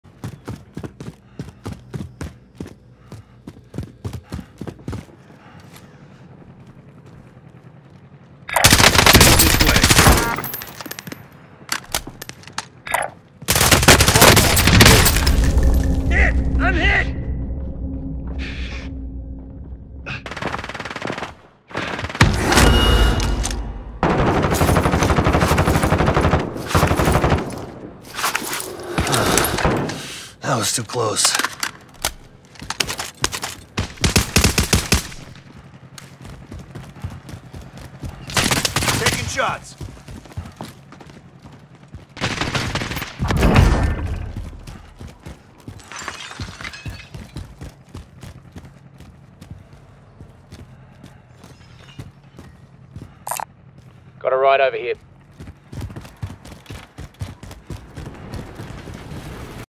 SDG Senzo gameplay quick kill.
Teams from two directions SDG Senzo getting a kill to start